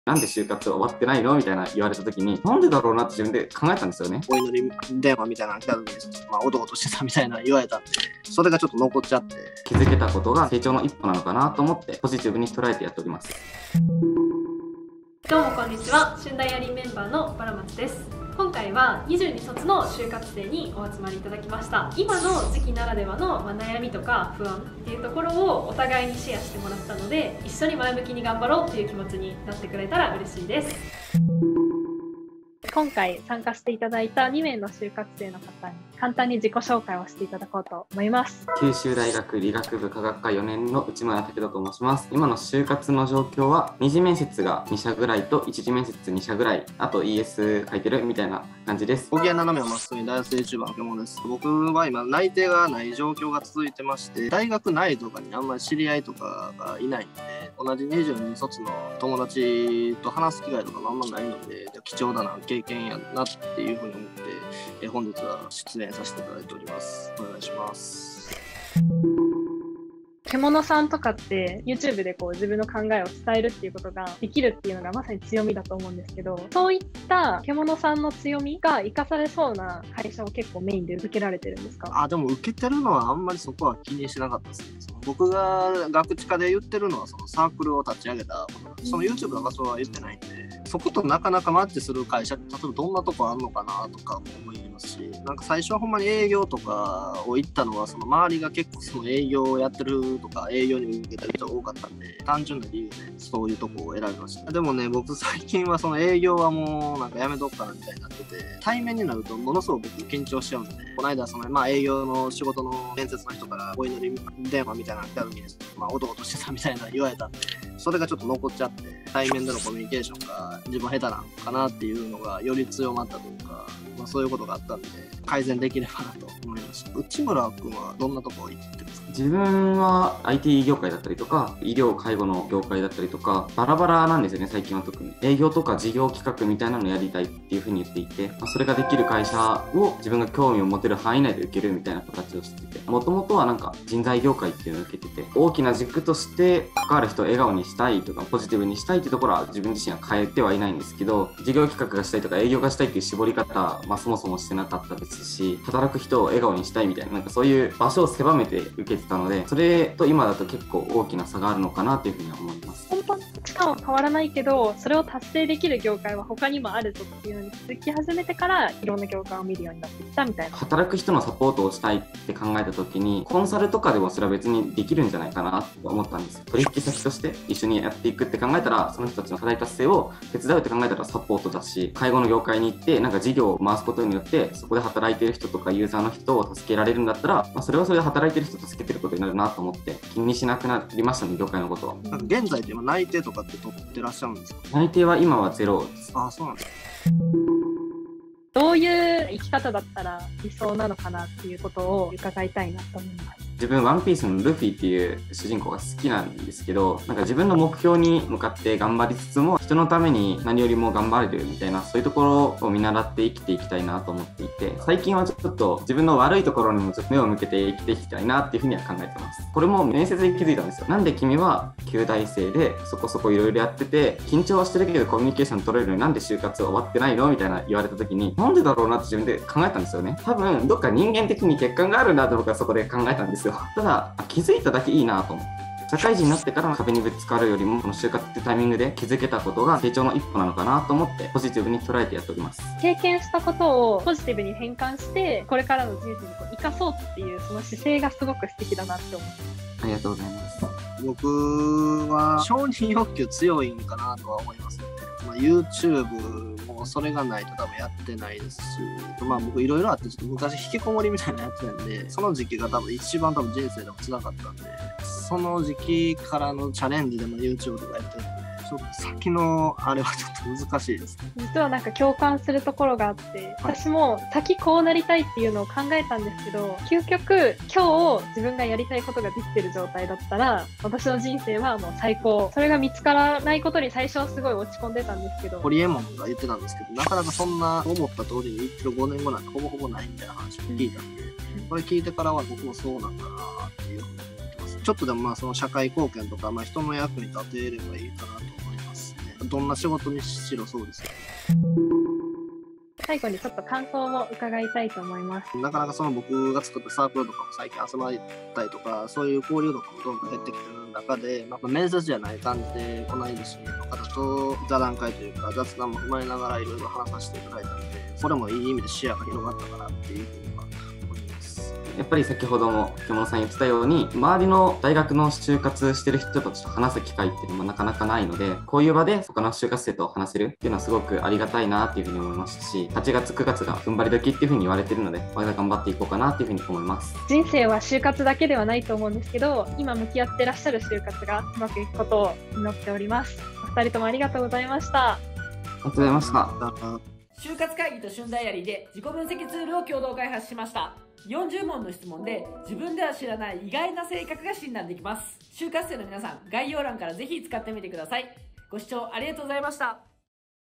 「持ち駒なくて不安...」大学4年のリアルすぎる就活事情
【内容】
0:14 企画説明
0:32 自己紹介
1:09 見る業界が変わった？
3:18 今後の人生をどう生きるか
5:02 就活だけが人生じゃない！

しゅんダイアリーと就活会議で共同開発した自己分析ツール第二弾はこちらから！

【就活会議とのオープンチャット開設しました】

ケモノくんのYouTubeチャンネルはこちら！

ケモノくんのTwitterはこちら！